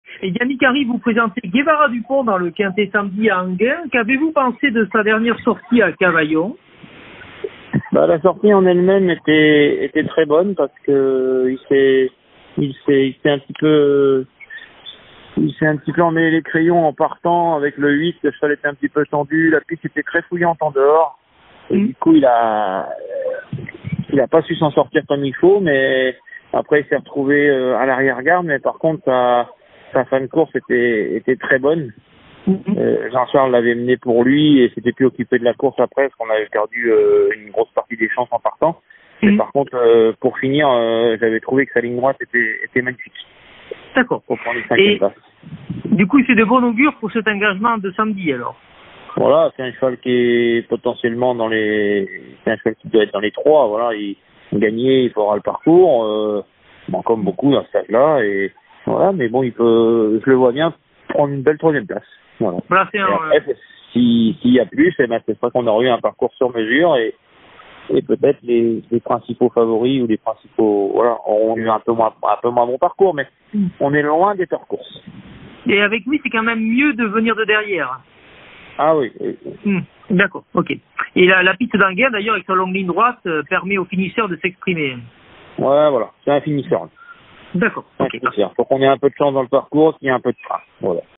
YANNICK HENRY | ENTRAÎNEUR DE GUÉVARA DU PONT | QUINTÉ DU SAMEDI 13 JUILLET À ENGHIEN
YANNICK HENRY, ENTRAÎNEUR DE GUÉVARA DU PONT (8) nous parle de ses chances dans le quinté du samedi 13 juillet 2024, le Prix du Palais de Chaillot sur l’hippodrome d'Enghien.